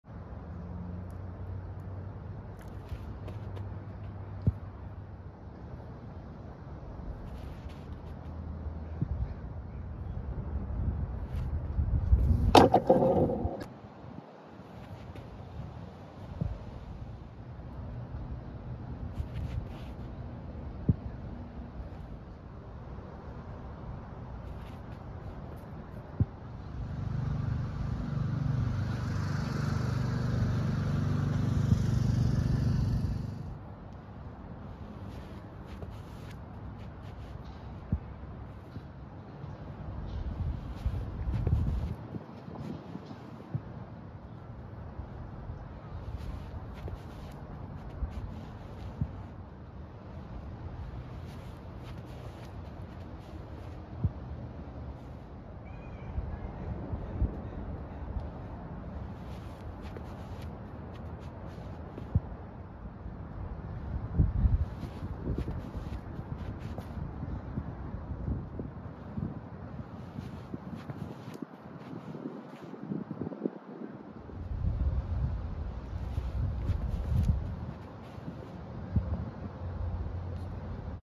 Braemar & Open Stone - 7.17.23
Braemar
 - x6, best ~41’

Open Stone
- x9, best 52’

Dealing with a ring finger issue. Fairly painful, feels like there isn’t much flick with the Braemar.